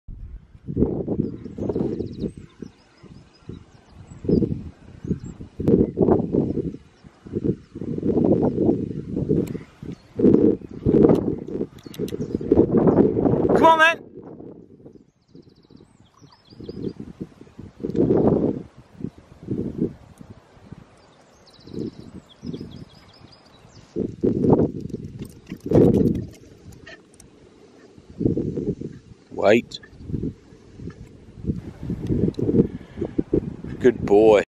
Badger 100m sit and stay. Fox Terrier
100M not bad for a stubborn wire haired fox terrier.